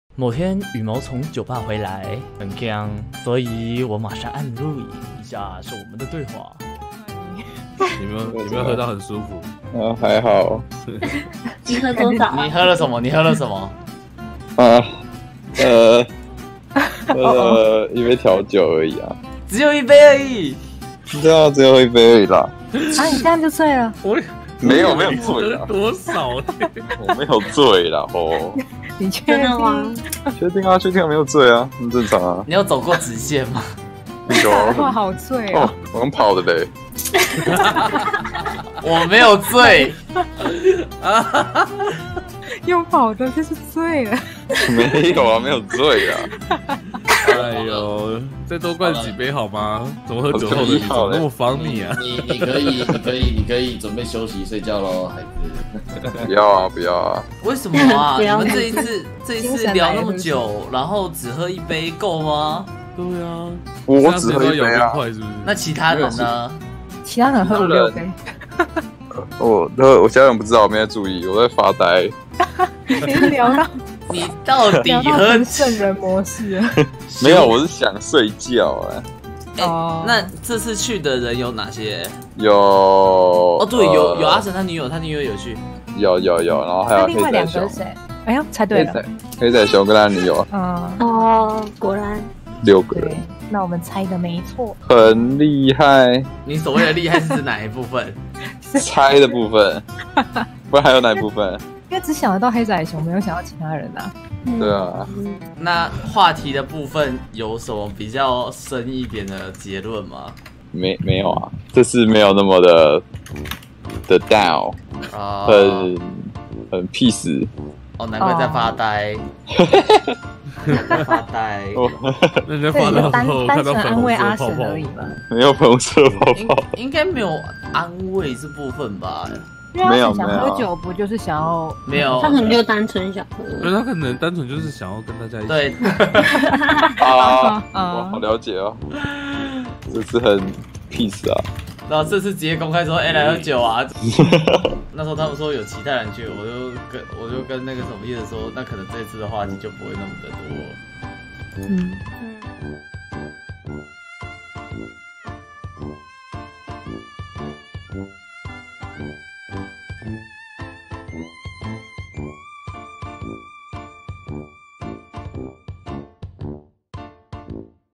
【夜生活】羽毛喝醉也太可愛(´▽`ʃ♡ƪ) 羽毛：我才沒有醉！我直線還用跑的(ﾟ∀。) 這不就喝醉了嗎(ﾉ∀`*) @shygolone @BoShen @showcome 葉紙 天空貓
這麼可愛的羽毛大概只能抱緊處理吧？